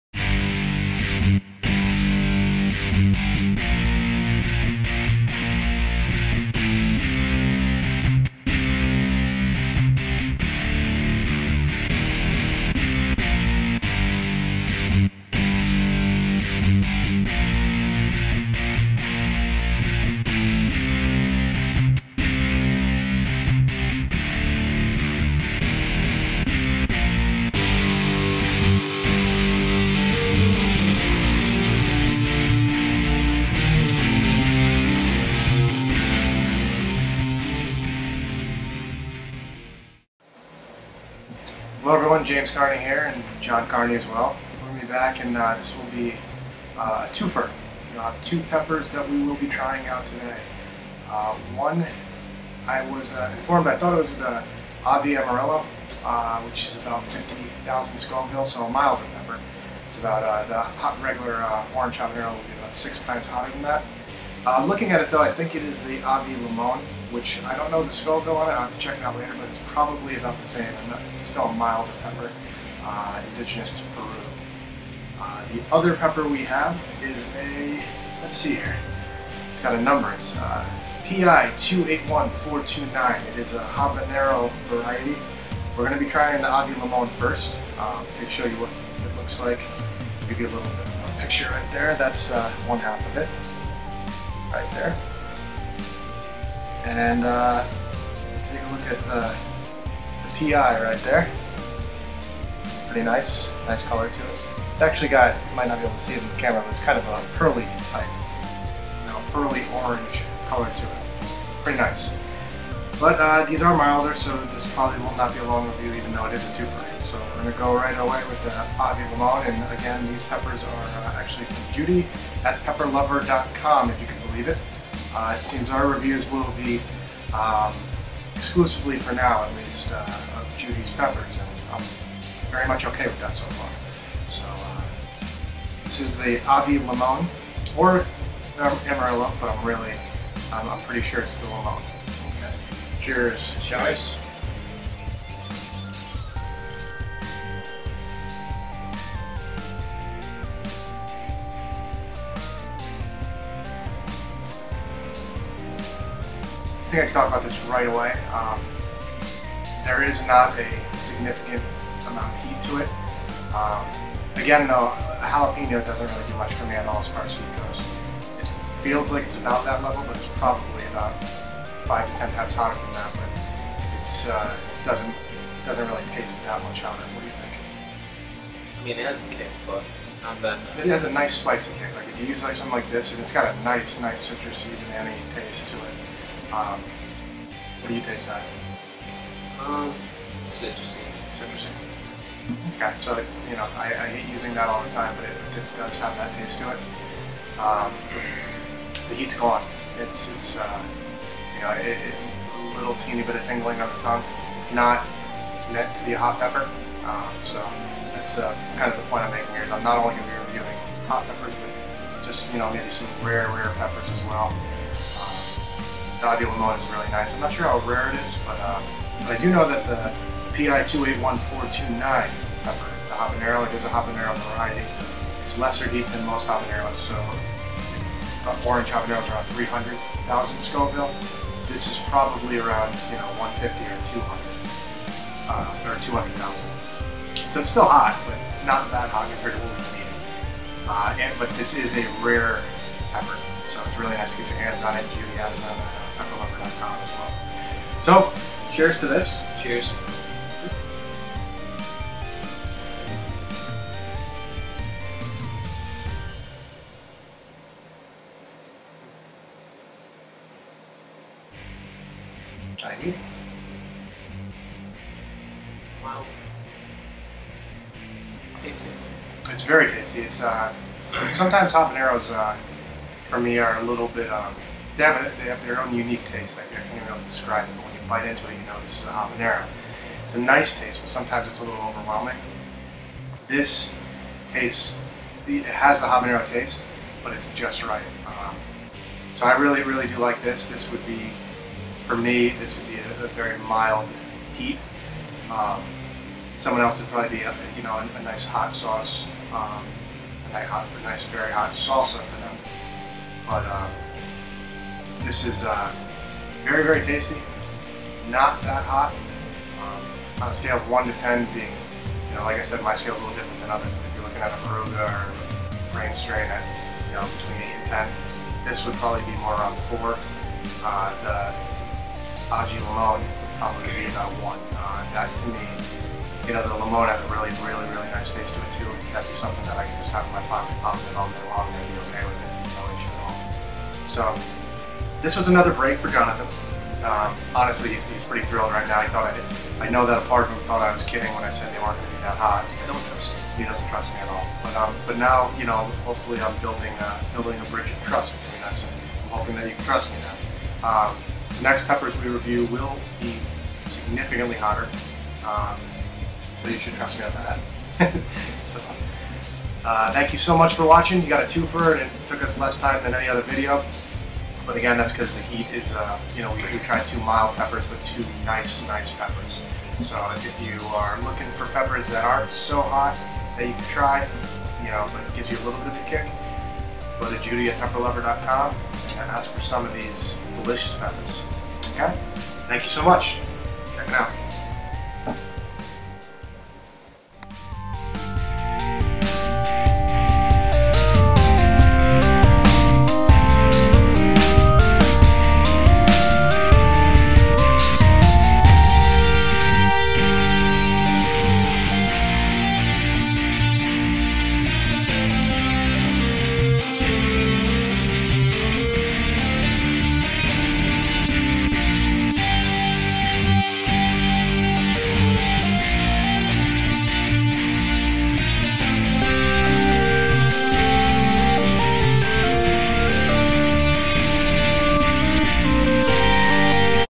Aji Limon and PI281429 Habanero Pepper Reviews (PEPPERLOVER.COM)
These were very tasty mild peppers.  Thank you so much Judy from PEPPERLOVER.COM.

Great seeds and great prices.